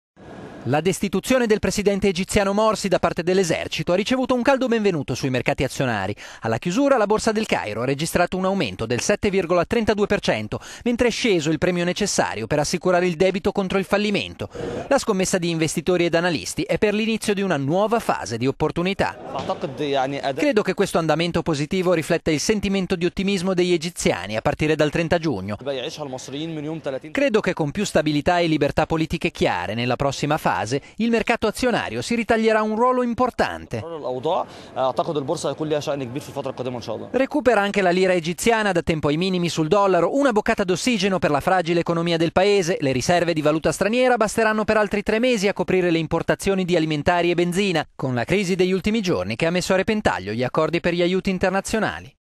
Egitto, la borsa festeggia dopo la destituzione di Morsi - economy
La destituzione del presidente egiziano Mohamed Morsi da parte dell'esercito ha ricevuto un caldo...

La destituzione del presidente egiziano Mohamed Morsi da parte dell'esercito ha ricevuto un caldo benvenuto sui mercati azionari.

Alla chiusura della seduta, questo giovedì, la Borsa del Cairo ha registrato un aumento del 7,32% mentre è sceso il prezzo dei credit default swap, cioè il premio necessario per assicurare il debito contro il fallimento.

La scommessa di investitori e analisti è per l'inizio di una nuova fase di opportunità: "Credo che questo andamento positivo rifletta il sentimento di ottimismo degli egiziani nato dopo il 30 giugno", spiega Amir Yousef, operatore di borsa.

"Penso che, con più stabilità e libertà politiche chiare nelle prossima fase, il mercato azionario si ritaglierà un ruolo importante".

In recupero anche la lira egiziana, che da tempo viaggiava ai minimi storici sul dollaro. Una boccata di ossigeno per la fragile economia del Paese: le riserve di valuta straniera basteranno per altri tre mesi a coprire le importazioni di alimentari e benzina.